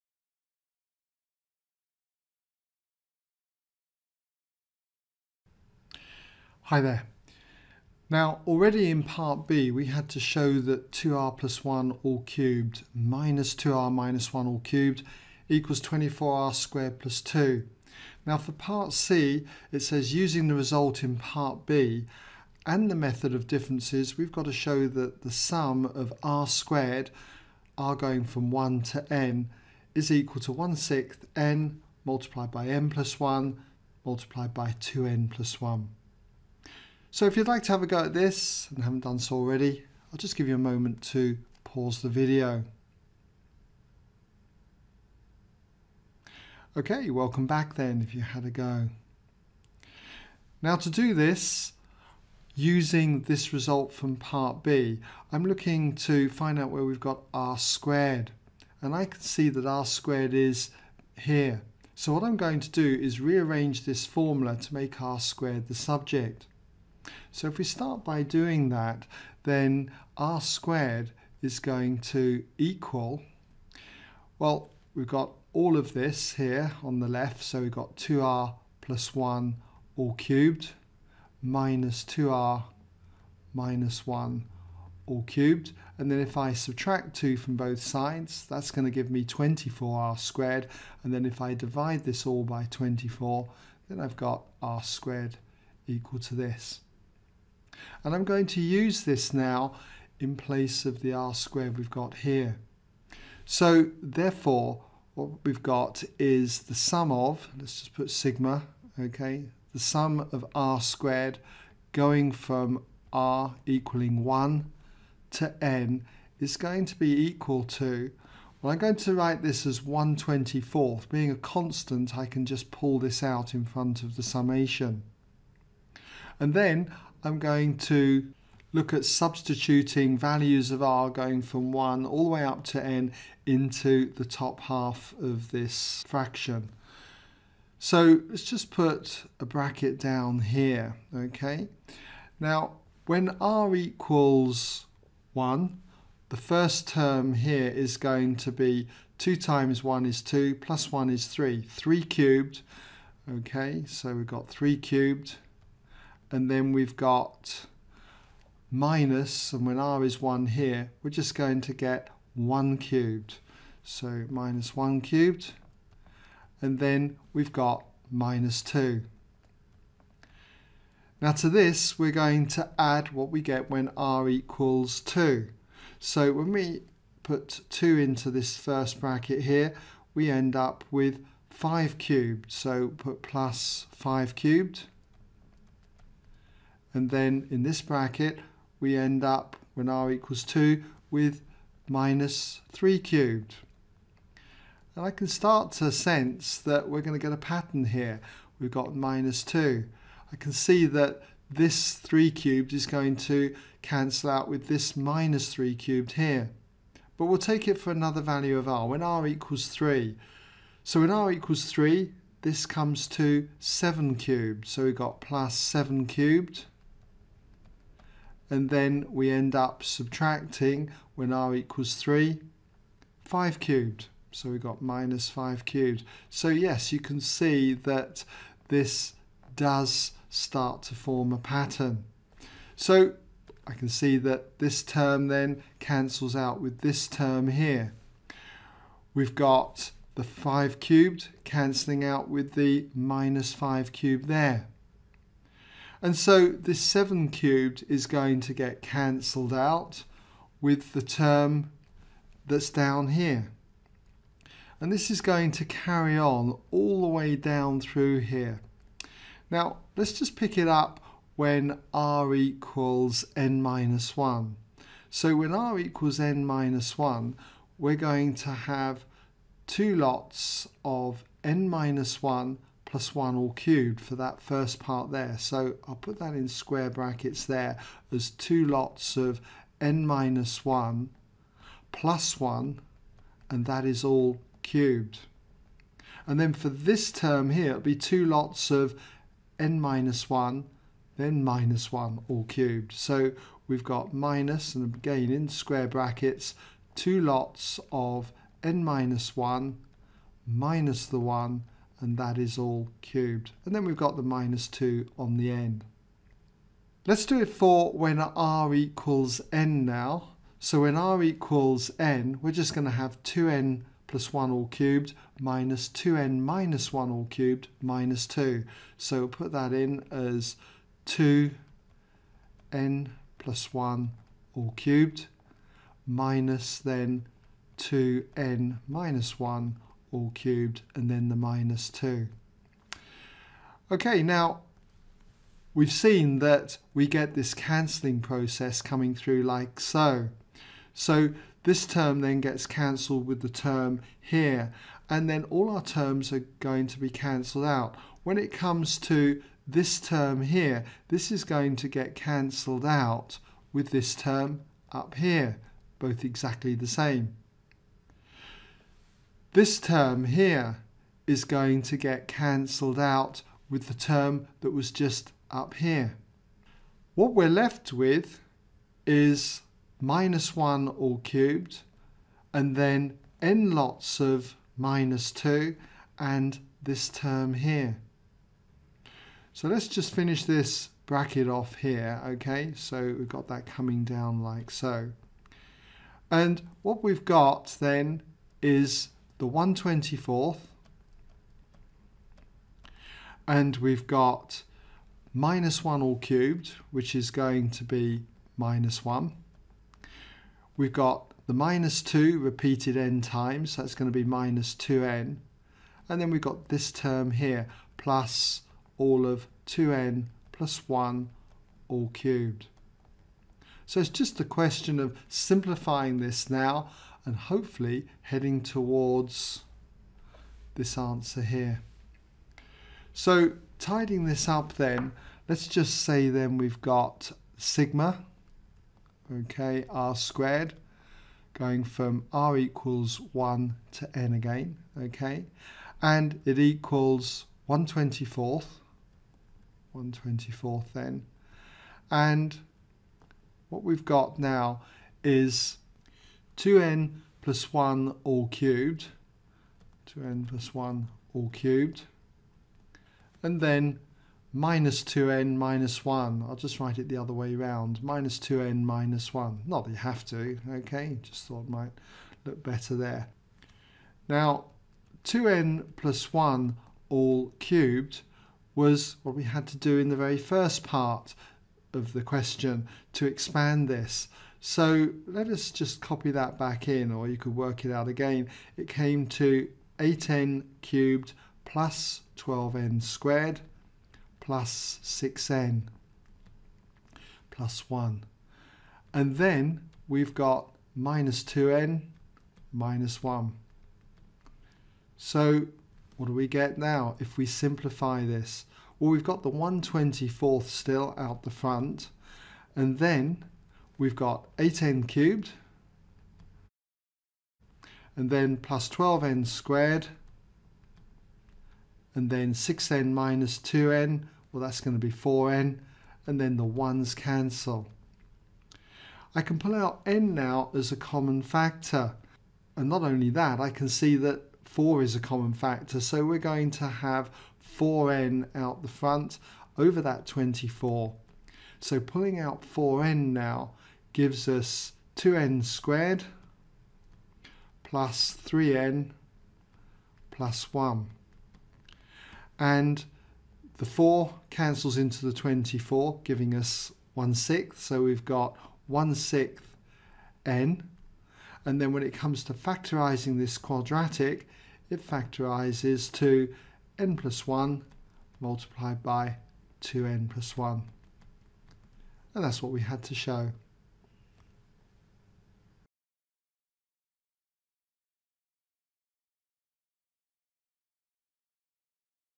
Summation using the method of differences : FP2 Edexcel June 2011 Q4(c) : ExamSolutions Maths
for the index, playlists and more maths videos on the method of differences and other maths topics.